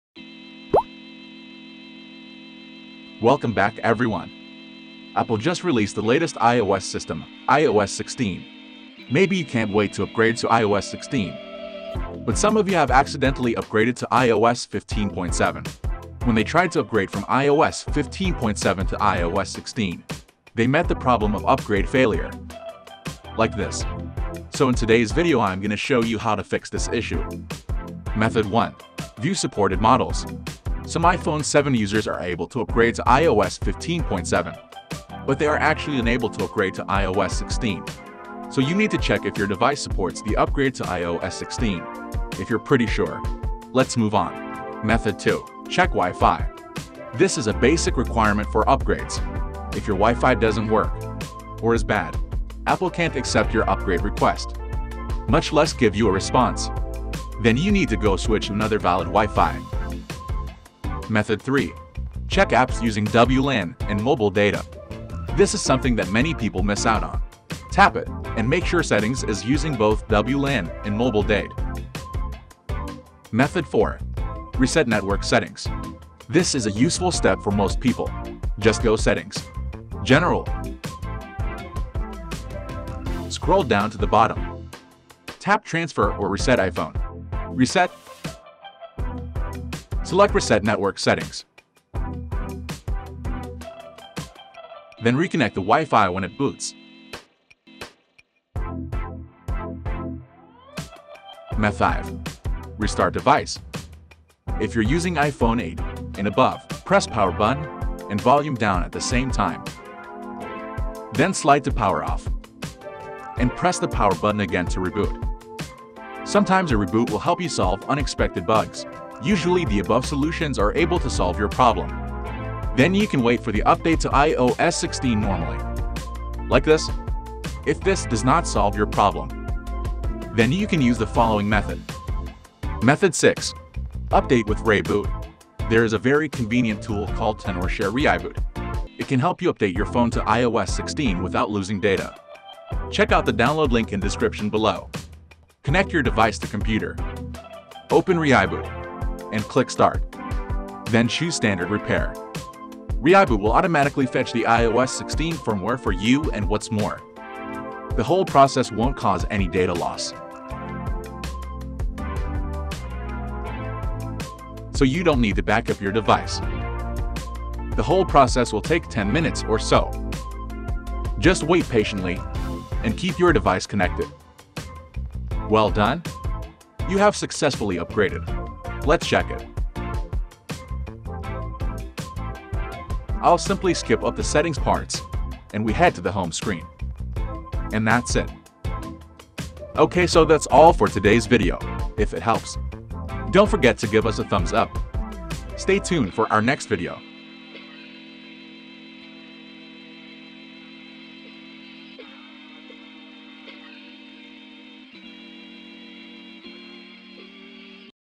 Can't update from iOS 17 to iOS 18? Software Update Failed?
Can't update from iOS 17 to iOS 18?  Software Update Failed? 6 Ways to help you fix it.
Unable to install update an error occurred installing ios 18?

Timestamps:
00:00 Introduction and preview
00:29 Method 1: View supported models
00:45 Method 2: Check Wi-Fi
01:02 Method 3: Check Apps Using WLAN and Mobile Data
01:14 Method 4: Reset network settings
01:42 Method 5: Restart Device
02:14 Method 6: Update with ReiBoot